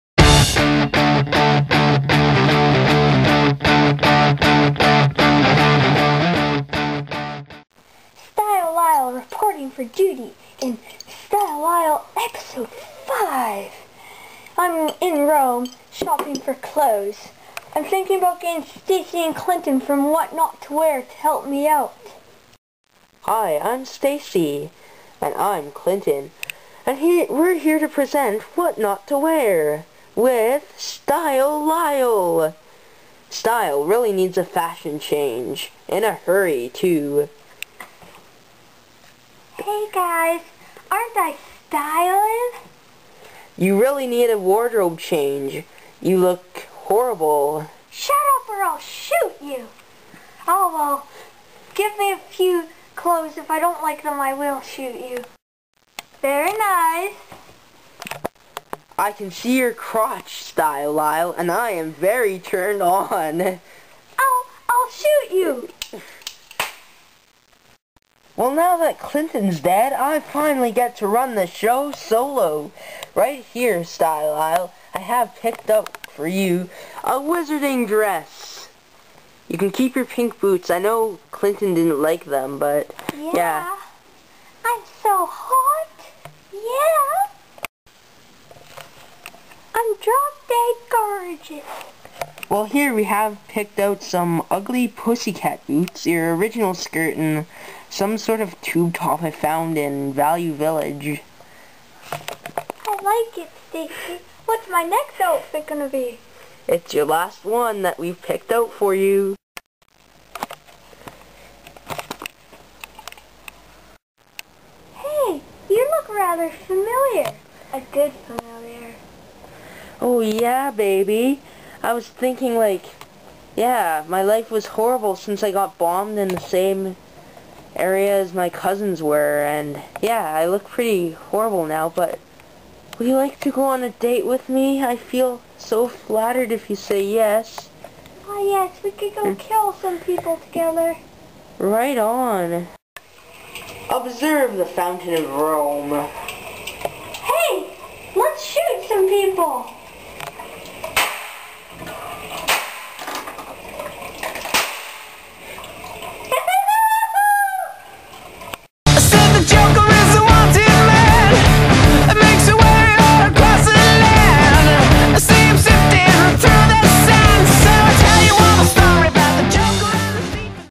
Style Lyle 5
style lyle goes on to what not to wear and gets a girlfriend.